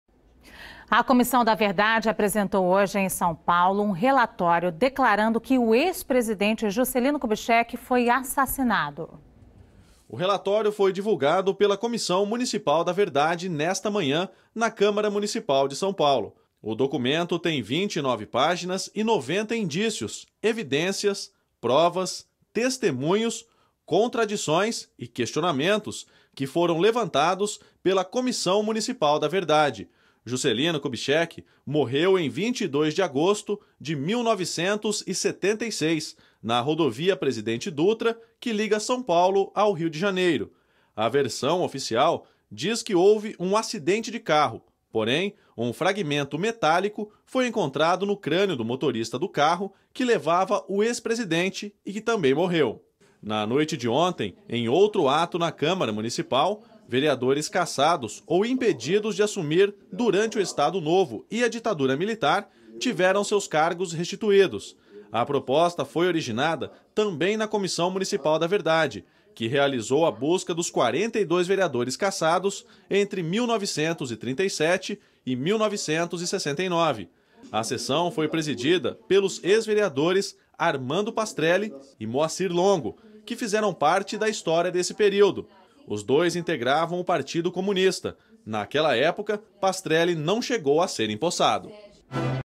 Comissão da Verdade apresenta relatório declarando que ex-presidente Juscelino Kubitschek foi a...
Versão oficial é de que a colisão entre o carro de JK e um ônibus na Via Dutra, em 22 de agosto de 1976, teria sido um acidente